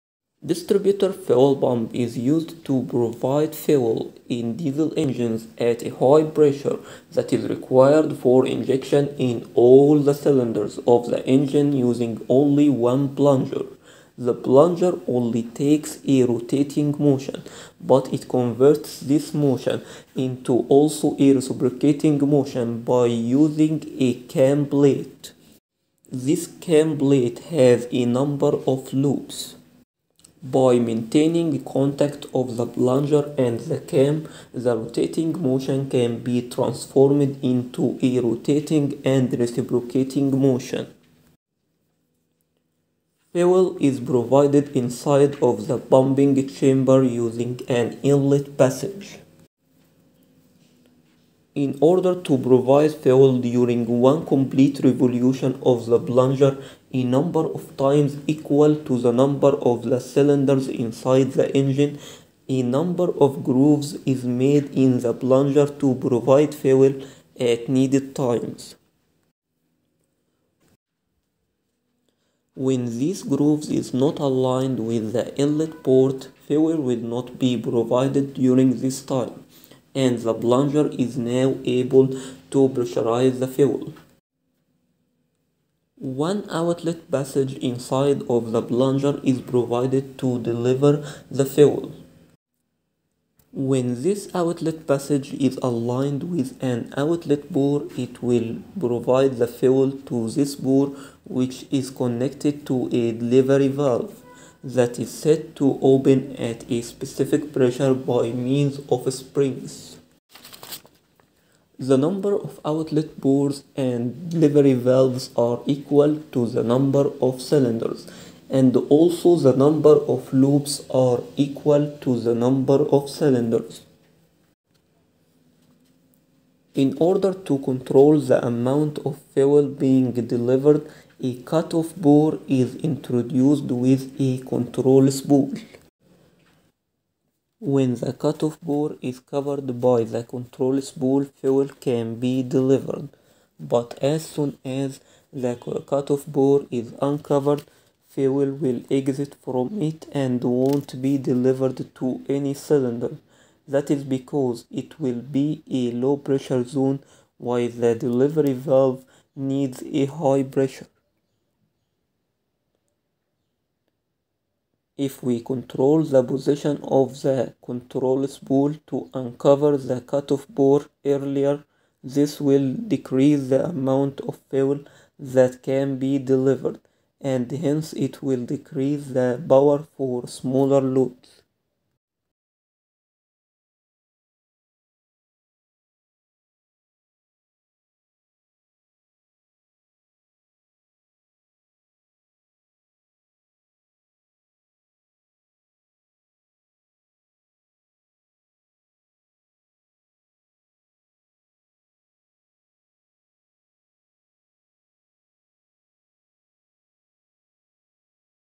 Distributor fuel pump (3d Animation)
Distributor fuel pump is used to provide the fuel to the engine cylinder at the required pressures
this animation is made entirely in blender 3d free software